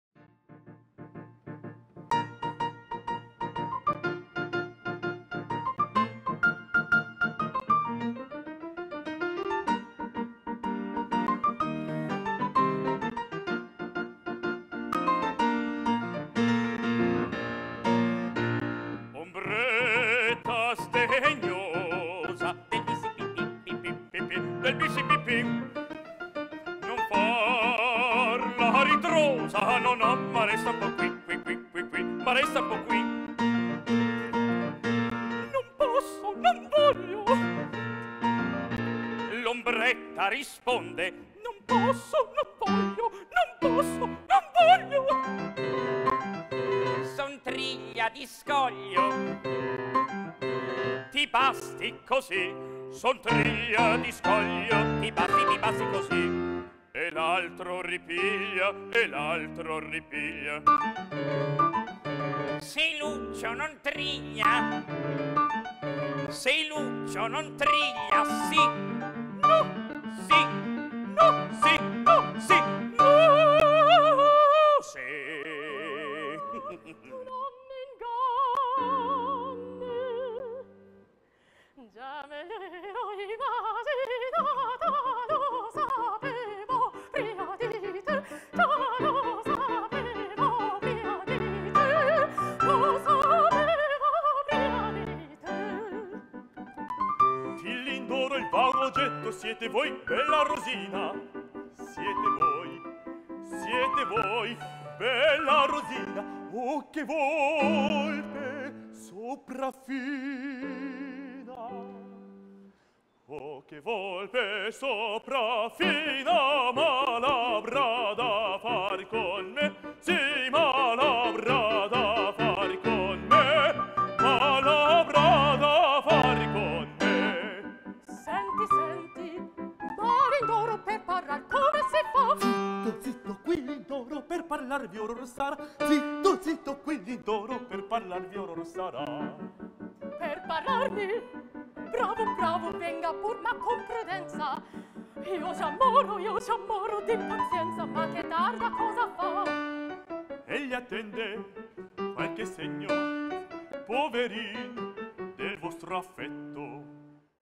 L'Opera buffa | Sharing Arts
Il baritono Giovanni Romeo, il mezzosoprano Aya Wakizono e il baritono Kwanghyun Kim, artisti dell'Accademia del Teatro alla Scala, di nuovo a THE WATERSTONE con l'interpretazione di alcune opere di Gioachino Rossini.